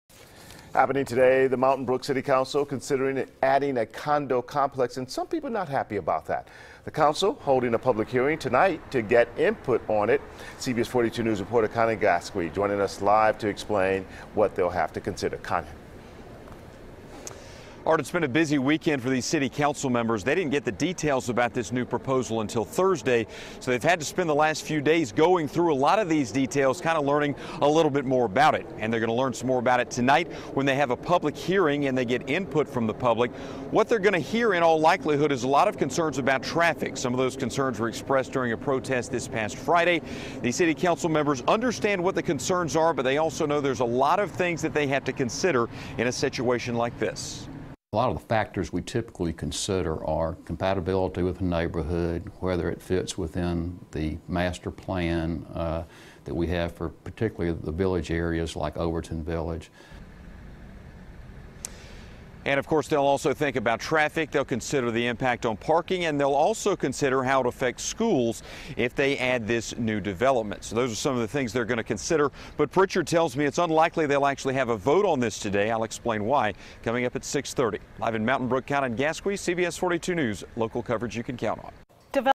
Mountain Brook considering development
Public hearing set for tonight at 7 p.m.